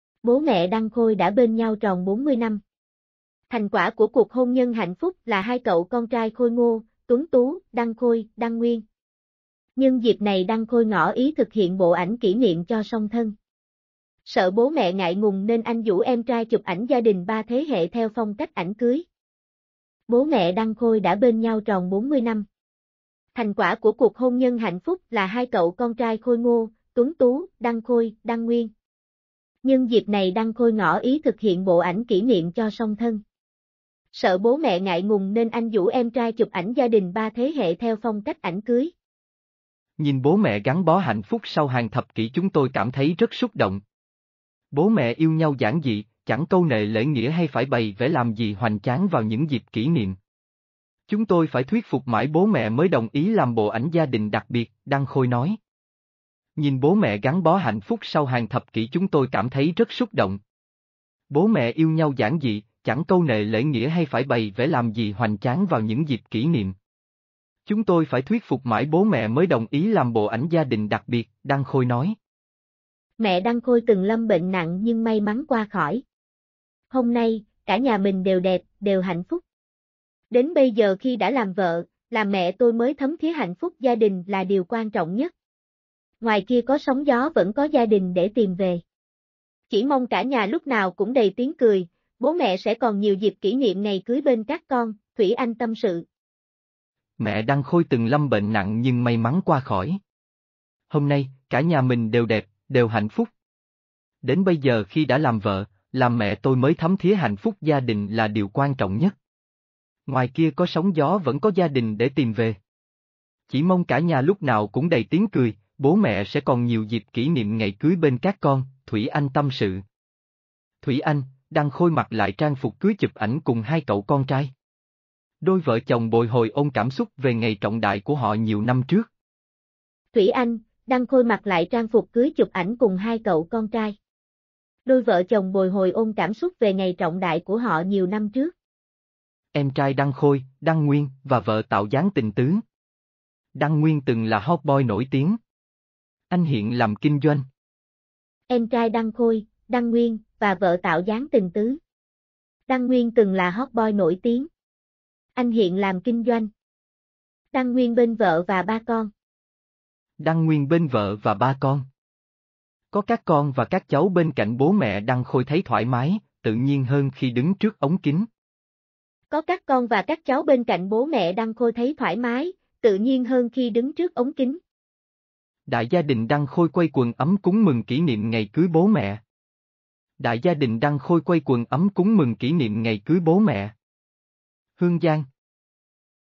Vợ chồng Đăng Khôi mừng 40 năm ngày cưới bố mẹ
Bố mẹ Đăng Khôi đã bên nhau tròn 40 năm. Thành quả của cuộc hôn nhân hạnh phúc là hai cậu con trai khôi ngô, tuấn tú: Đăng Khôi, Đăng Nguyên. Nhân dịp này Đăng Khôi ngỏ ý thực hiện bộ ảnh kỷ niệm cho song thân. Sợ bố mẹ ngại ngùng nên anh rủ em trai chụp ảnh gia đình ba thế hệ theo phong cách ảnh cưới.
'Nhìn bố mẹ gắn bó hạnh phúc sau hàng thập kỷ chúng tôi cảm thấy rất xúc động. Bố mẹ yêu nhau giản dị...